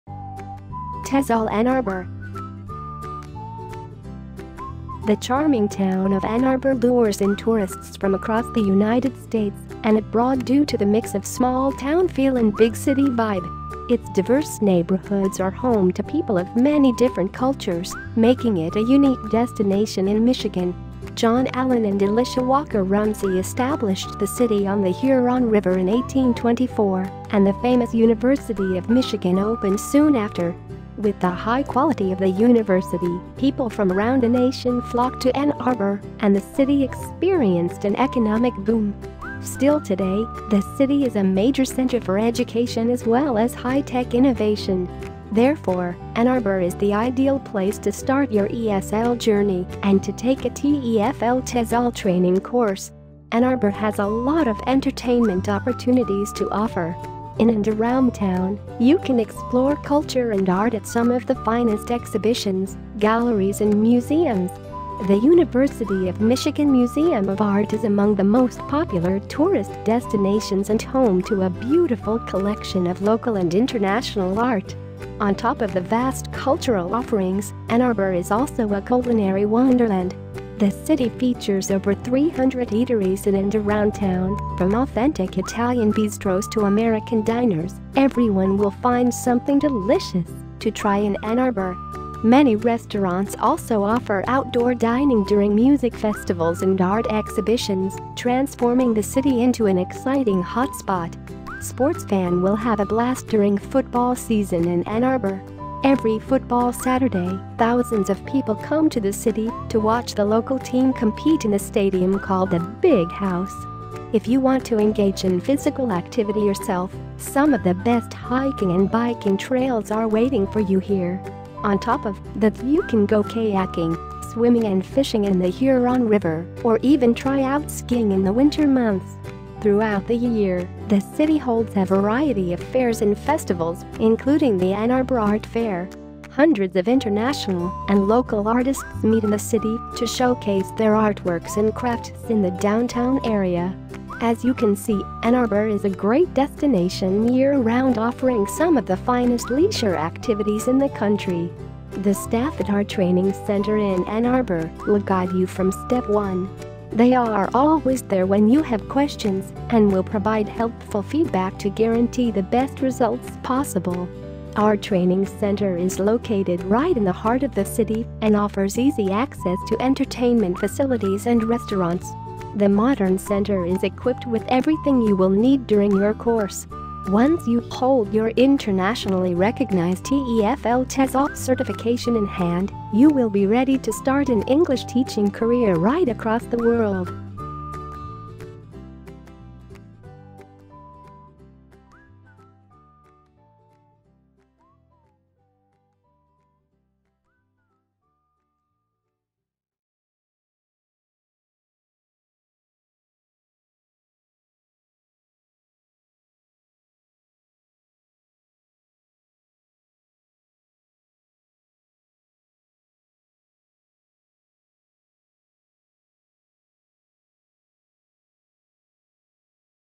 Combined TEFL / TESOL Course in Ann Arbor, USA | Teach & Live abroad!
The charming town of Ann Arbor lures in tourists from across the united states and abroad due to the mix of small town feel and big city vibe. Its diverse neighborhoods are home to people of many different cultures, making it a unique destination in Michigan. John Allen and Elisha Walker Rumsey established the city on the Huron River in 1824 and the famous University of Michigan opened soon after. With the high quality of the university, people from around the nation flocked to Ann Arbor and the city experienced an economic boom. Still today, the city is a major center for education as well as high-tech innovation. Therefore, Ann Arbor is the ideal place to start your ESL journey and to take a TEFL/TESOL training course.

Ann Arbor has a lot of entertainment opportunities to offer. In and around town, you can explore culture and art at some of the finest exhibitions, galleries and museums. The University of Michigan Museum of Art is among the most popular tourist destinations and home to a beautiful collection of local and international art. On top of the vast cultural offerings, Ann Arbor is also a culinary wonderland. The city features over 300 eateries in and around town, from authentic Italian bistros to American diners, everyone will find something delicious to try in Ann Arbor. Many restaurants also offer outdoor dining during music festivals and art exhibitions, transforming the city into an exciting hotspot.

Sports fan will have a blast during football season in Ann Arbor. Every football Saturday, thousands of people come to the city to watch the local team compete in the stadium called “The Big House”. If you want to engage in physical activity yourself, some of the best hiking and biking trails are waiting for you here. On top of that you can go kayaking, swimming and fishing in the Huron River or even try out skiing in the winter months. Throughout the year, the city holds a variety of fairs and festivals, including the Ann Arbor Art Fair. Hundreds of international and local artists meet in the city to showcase their artworks and crafts in the downtown area. As you can see, Ann Arbor is a great destination year-round offering some of the finest leisure activities in the country.

The staff at our training center in Ann Arbor will guide you from step one. They are always there when you have questions and will provide helpful feedback to guarantee the best results possible. Our training center is located right in the heart of the city and offers easy access to entertainment facilities and restaurants. The modern center is equipped with everything you will need during your course. Once you hold your internationally recognized TEFL/TESOL certification in hand, you will be ready to start an English teaching career right across the world.

Find out more about taking a TEFL course in Orange County